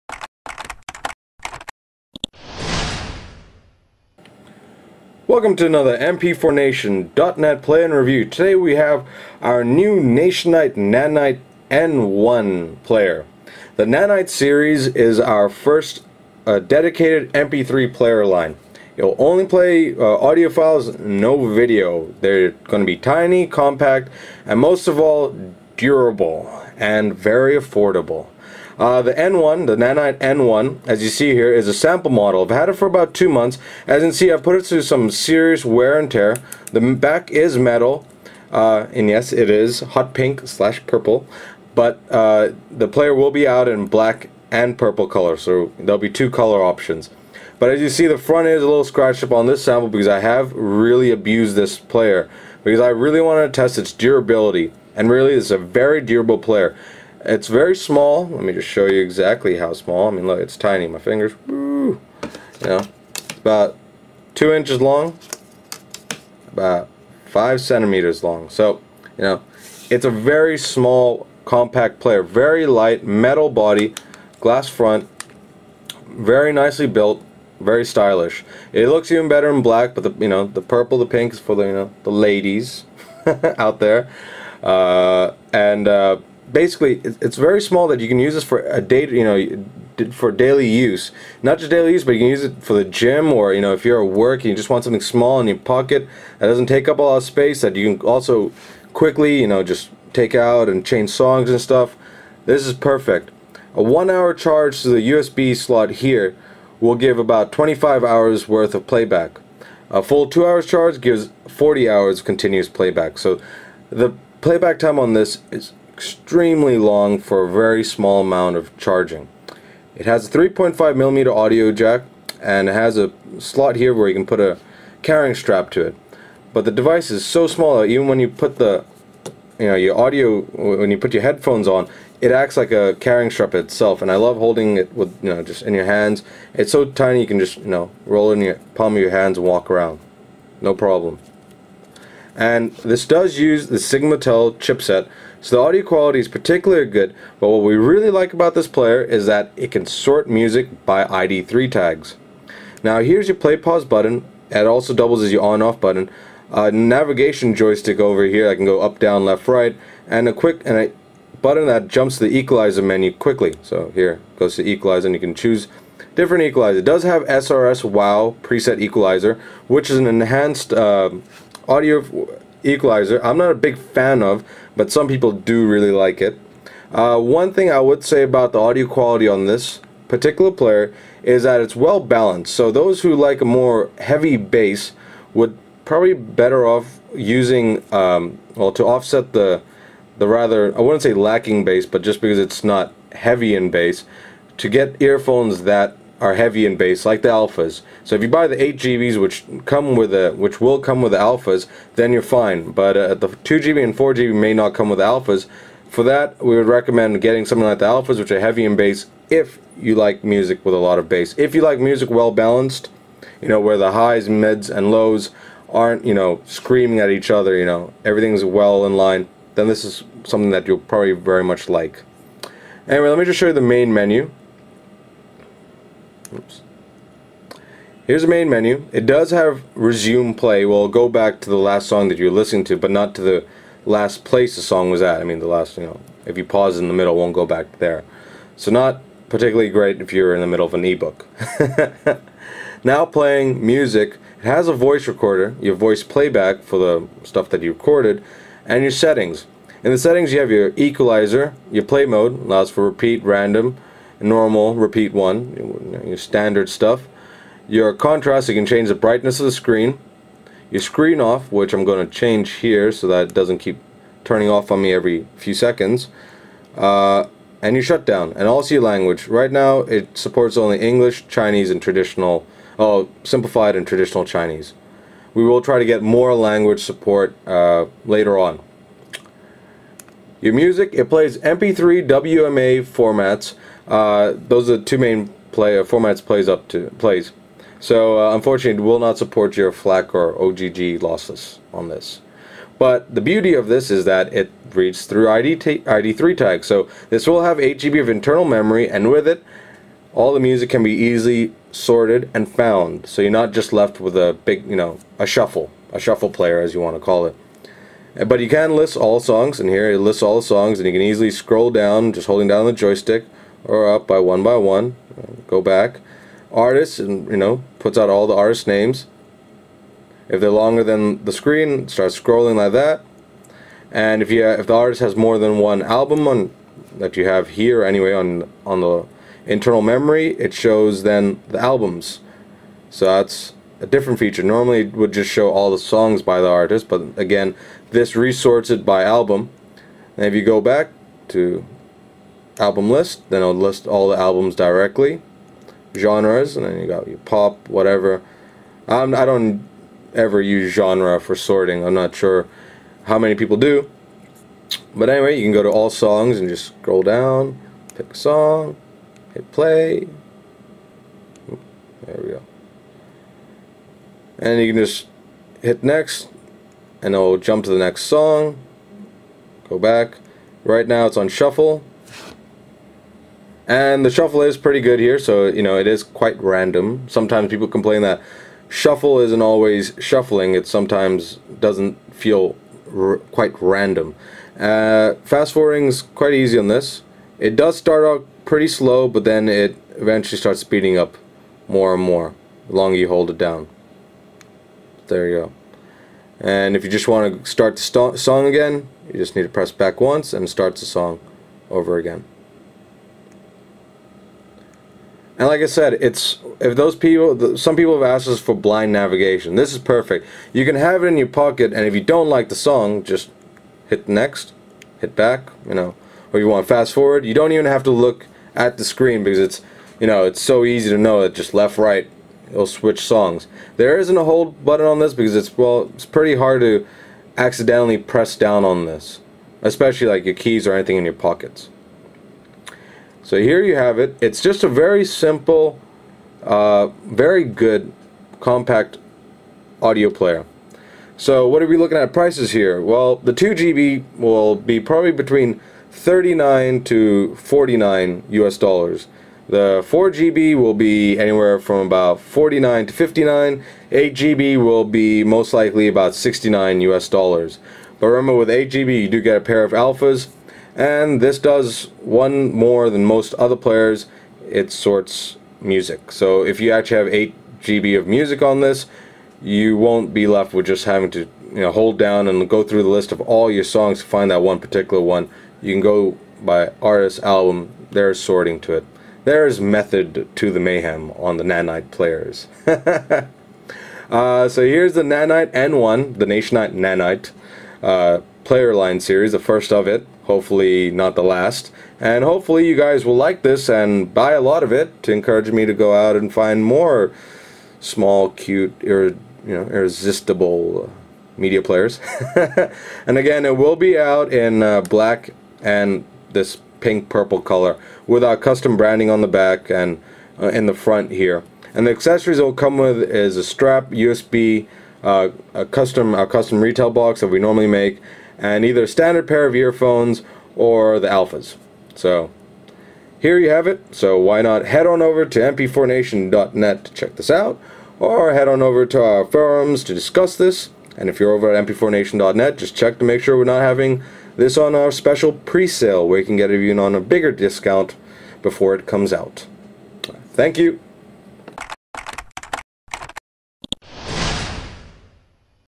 Nationite NaNite N1 MP3 Player - Small Compact and durable media player
The Nationite NaNite N1 is the first player in our new NaNite MP3 player line, it has a small compact design (only 5cm in length), uses the Sigmatel chipset, which allows for good audio quality and ID3 tag sorting.

This is a sample model and the actual model will be out by the second week of July 2009.

The small player will come in 2GB, 4GB and 8GB of internal memory, it will come in Black and Pink color options.